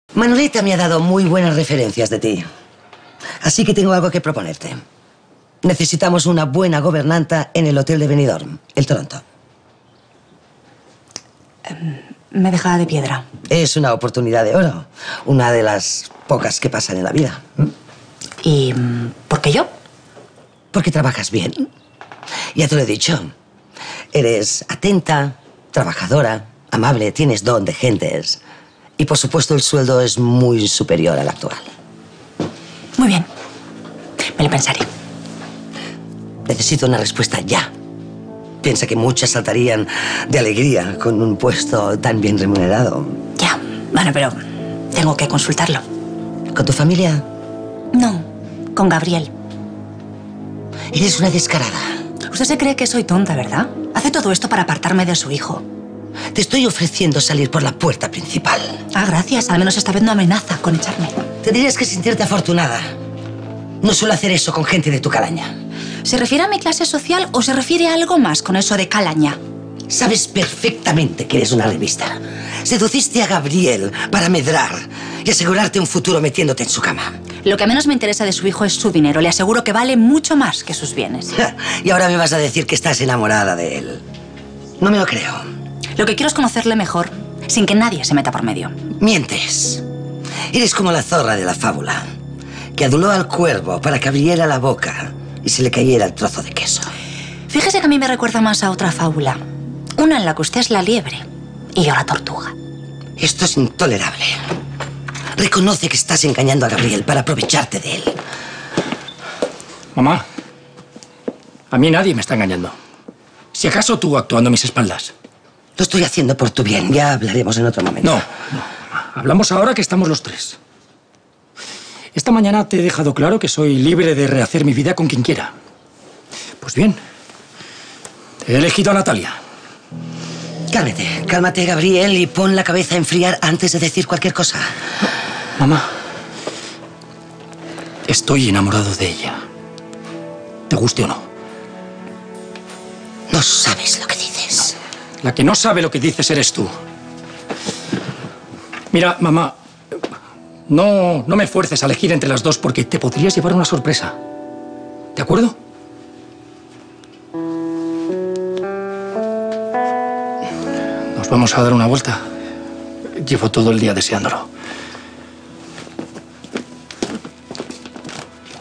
Gabriel defiende a Natalia tras discutir con Ascensión: "Estoy enamorado de ella"
Ascensión ofrece a Natalia el puesto de gobernanta en su hotel de Benidorm, pero no tiene tiempo para pensárselo, Ascensión necesita una respuesta rápida. Natalia sabe la razón por la que le ofrece el puesto y no está dispuesta a que se deshaga de ella para conseguir que se aleje de Gabriel. Éste les sorprende discutiendo y no duda en defender a Natalia: "Estoy enamorado de ella, te guste o no".